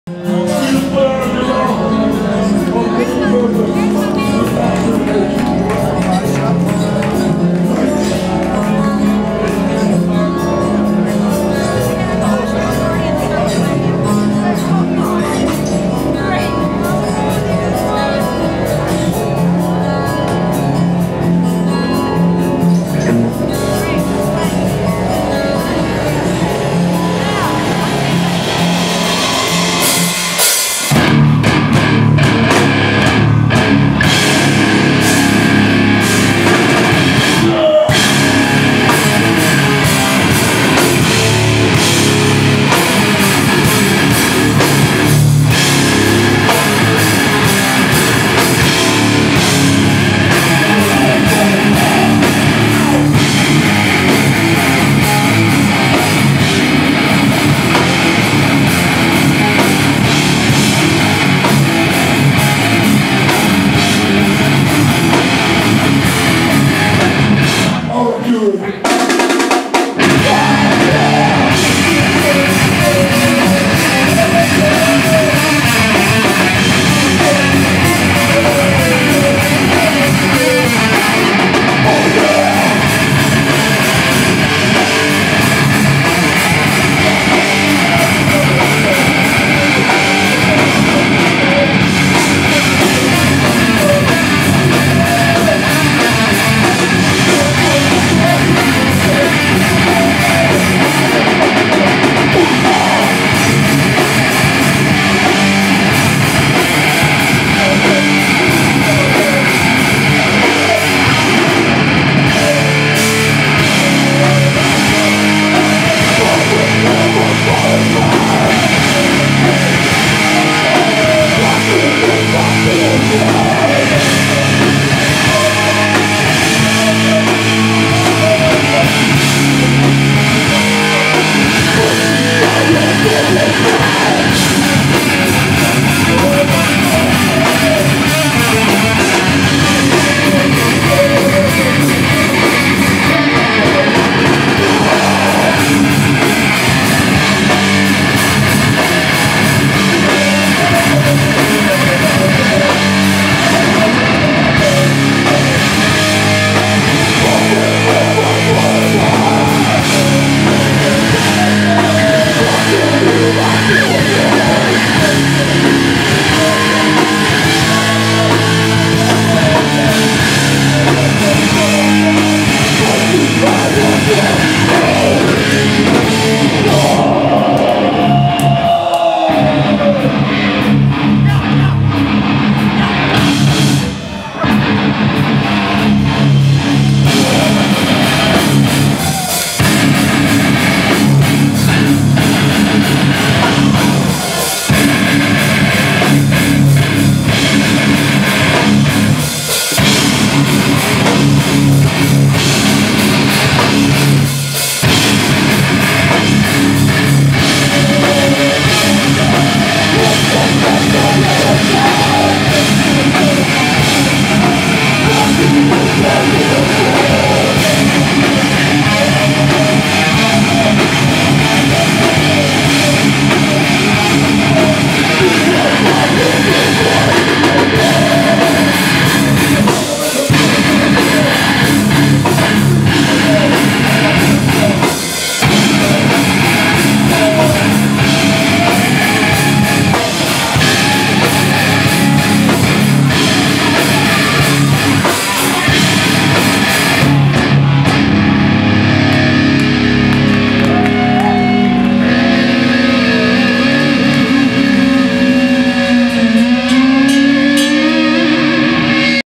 Twitching Tongues - United Blood 2018
United Blood 2018
Canal Club
Richmond VA